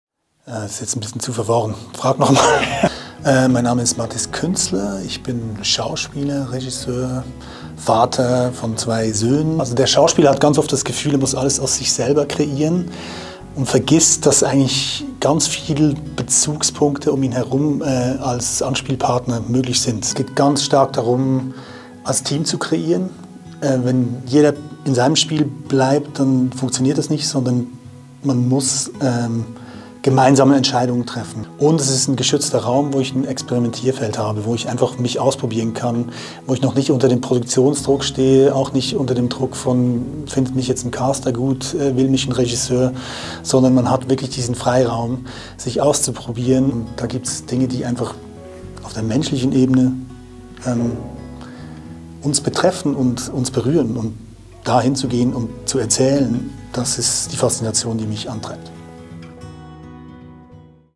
Mathis Künzler
Seit Januar 2023 Teil der Filmschauspielschule Zürich filmZ
Herzlich willkommen, Mathis Künzler
Dozent für Schauspieltechnik